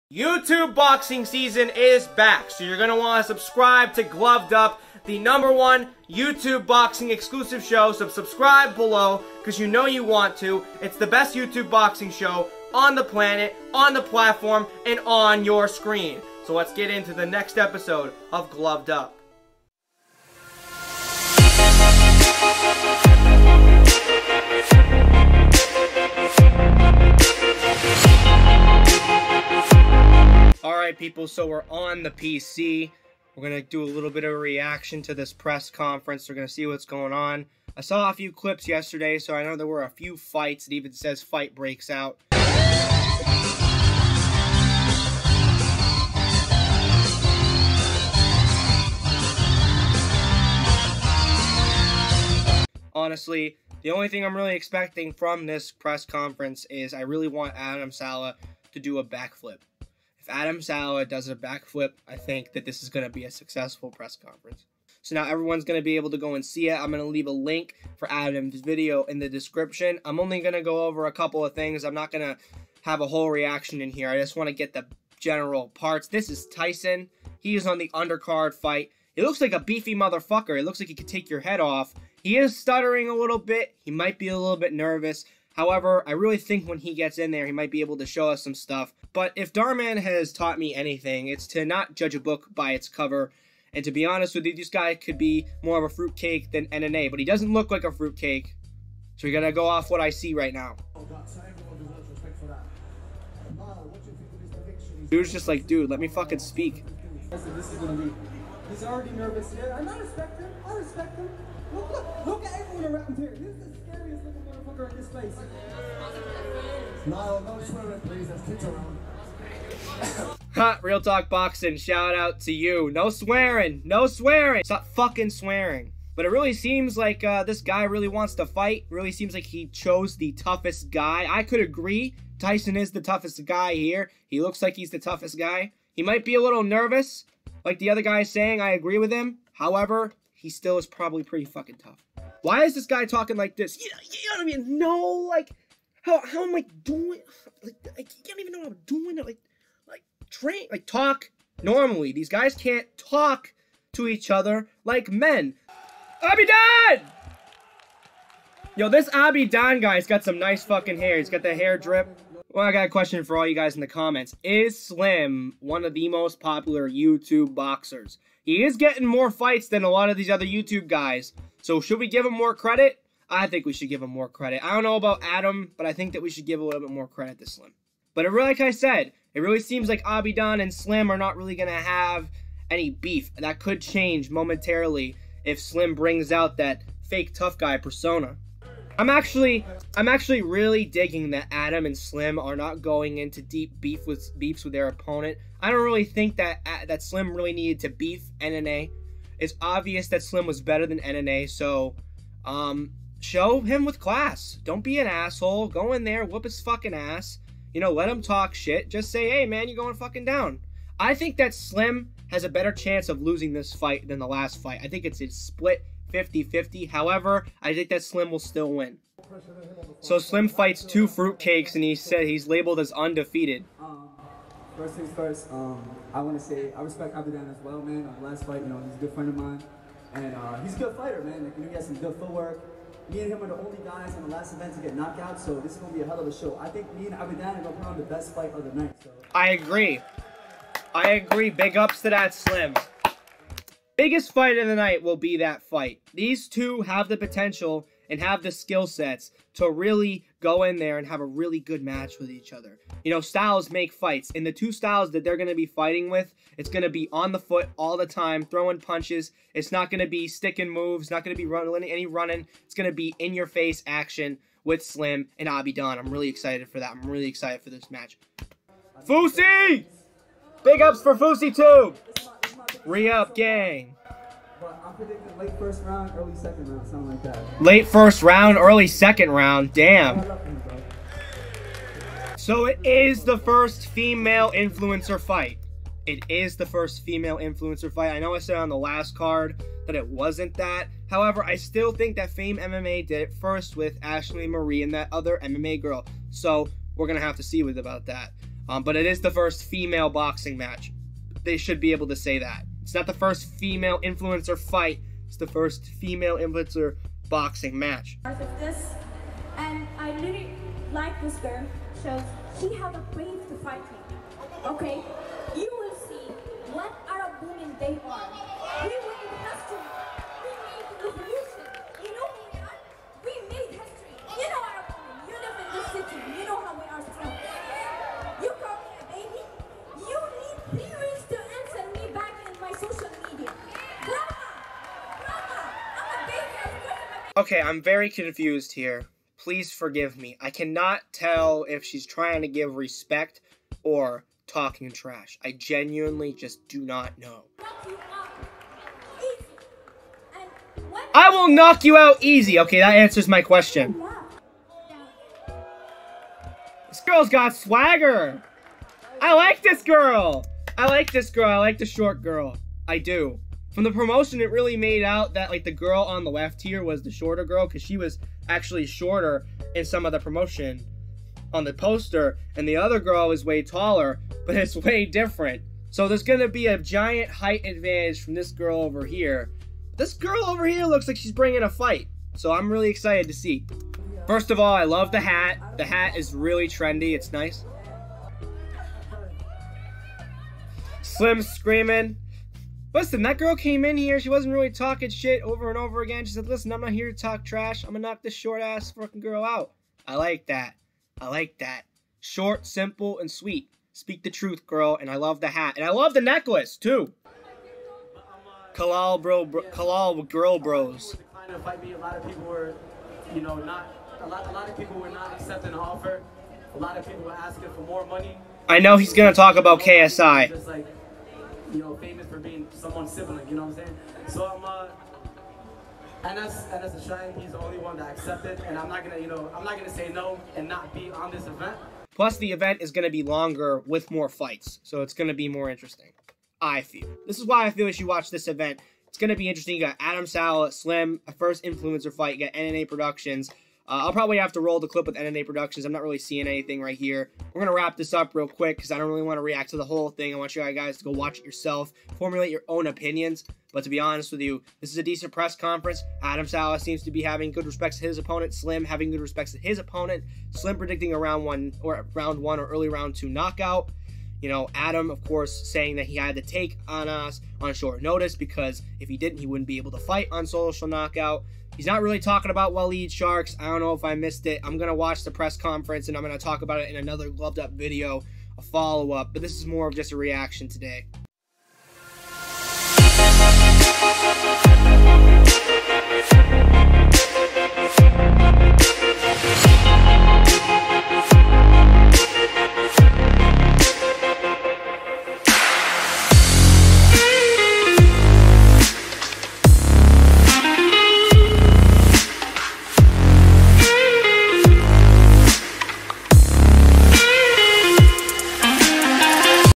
Social Knockout 2 Press Conference REACTION! (Fight breaks out) | Gloved Up CLIPS : Youtube Boxing
Koko swap is back for Social Knockout 2 as Anas takes on Adam Saleh, Slim returns to the ring against 1-0 3bidan. N and a returns as well along with the first female influencer boxing match! The Social Knockout 2 press conference has taken place and today we do a reaction

this is a clip from an upcoming episode